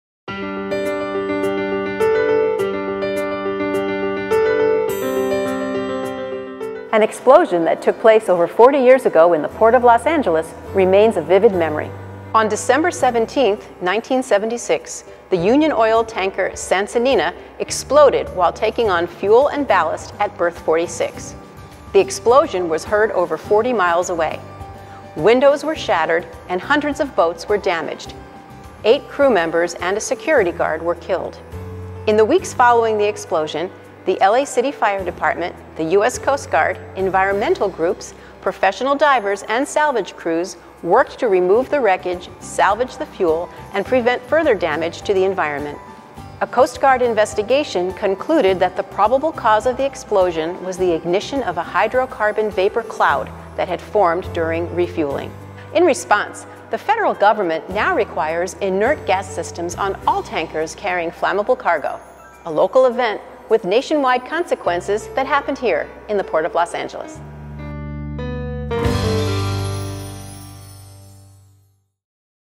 Maritime Minute: SS Sansinena
Some Los Angeles residents may remember an explosion that occurred at the Port of Los Angeles more than 40 years ago. In this episode of Maritime Minute, Marifrances Trivelli, Director of the Los Angeles Maritime Museum, describes how the oil tanker SS Sansinena exploded at dock, and how this event led to stricter federal requirements for these vessels.